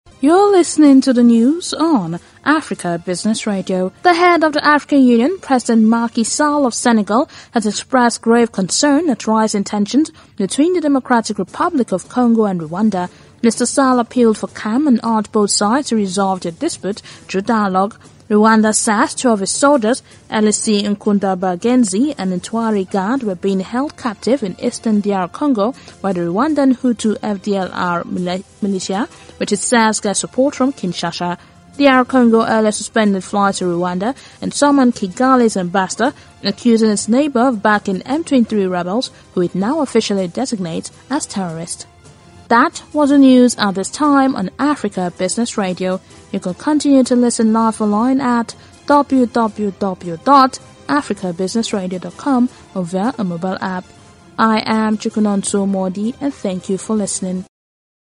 Rwanda-Dr Congo Diplomatic Tensions Spark Grave Concern
The head of the African Union, President Macky Sall of Senegal, has expressed grave concern at rising tensions between the Democratic Republic of Congo and Rwanda.
Mr Sall appealed for calm and urged both sides to resolve their dispute through dialogue.
Rwanda says two of its soldiers - Élysée Nkundabagenzi and Ntwari Gad - were being held captive in eastern DR Congo by the Rwandan Hutu FDLR militia, which it says gets support from Kinshasa.
DR Congo earlier suspended flights to Rwanda and summoned Kigali's ambassador - accusing its neighbour of backing M23 rebels, who it now officially designates as terrorists.